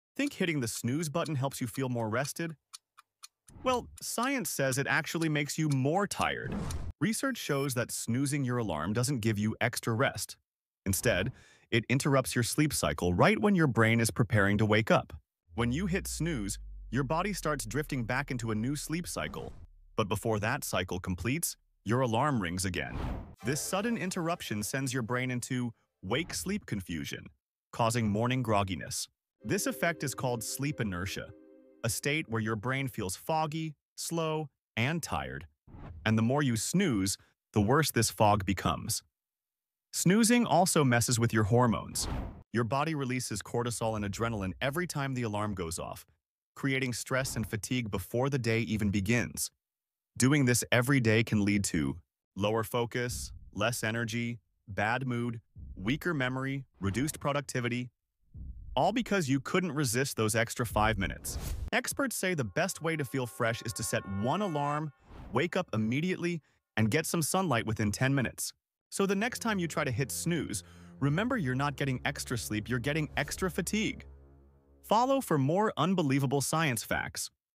Why Hitting Snooze Makes You Tired!
Think hitting the snooze button gives you extra rest? Think again! 💤 Science reveals that snoozing your alarm actually makes you feel MORE tired. It disrupts your sleep cycle, leading to morning grogginess and reduced productivity. Discover how sleep inertia affects your day and learn the best way to wake up refreshed! 🌞 Follow for more unbelievable science facts!